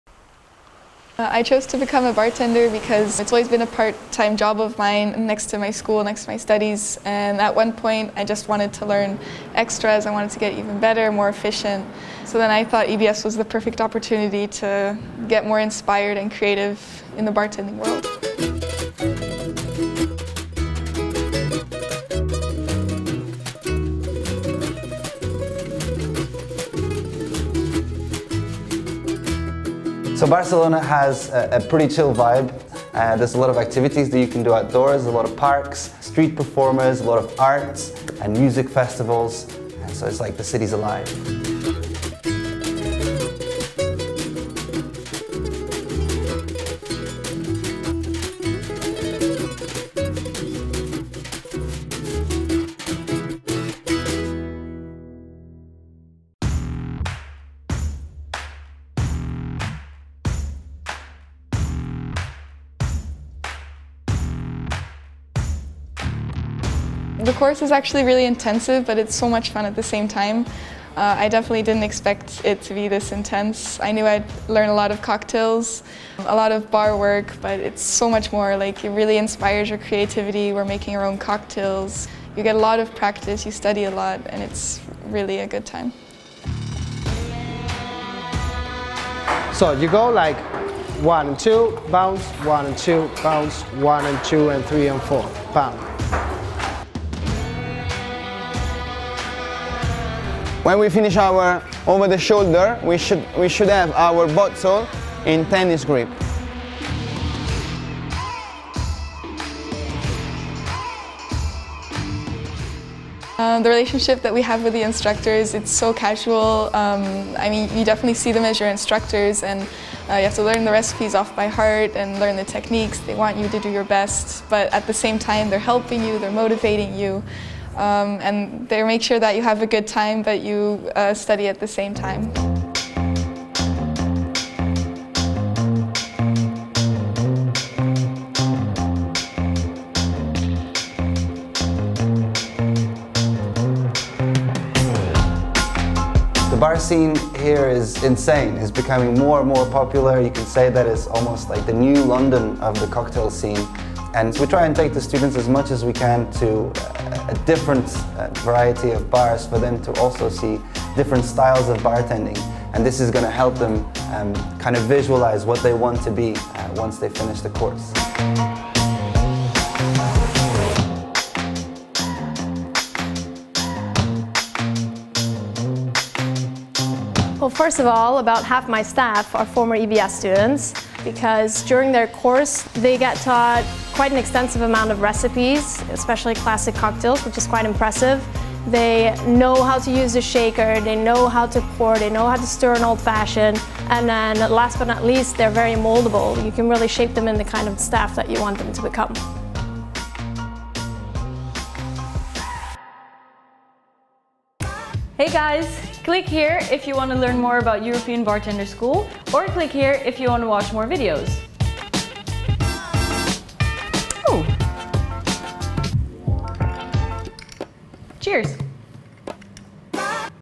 EBS Barcelona School | European Bartender School
Become a certified bartender in Barcelona with EBS Barcelona. Take our International Bartender Course and you’ll learn everything you need to become an efficient, skilful, and confident bartender.

Barcelona is located in the northeast of Spain, it is the capital and the largest city in Catalonia. It is one of Europe’s most cosmopolitan and stylish cities; young people from across the globe flock to Barcelona every year to soak up the sun and experience life in the city. So why not join them and take a bartender course with European Bartender School?

The Barcelona bartender school is located in the very heart of the city. It’s only a 2-minute walk from the iconic square of Plaza Catalunya. The school boasts 25 fully-equipped bar, free pouring stations, a live bar, a theory, and a flair area. The bartender school is designed so that you can learn as much as possible during your course. This means you’ll spend a lot of time behind the bar; making new cocktails and learning the practices of a modern bartender.

By joining our International Bartender Course with EBS Barcelona, you’ll be part of one of our most popular courses. The school offers a variety of professional bartender courses and also teach them in multiple languages.

Outside of the classroom, you’ll spend most of your time discovering the endless list of things to do. A perfect day would consist of walking from the school down Las Ramblas to the Barceloneta beach, then walking through the beautiful Ciutadella park, and finally, ending up in the bars of the magical district of El Gotico. One must-do for everyone who visits the city is to head up to Los Bunkers and take in the astonishing panoramic views of the city.

Why not take advantage of the city’s booming bartending scene and search for a job after graduating from your International Bartender Course? Barcelona is the city with style that you just have to experience.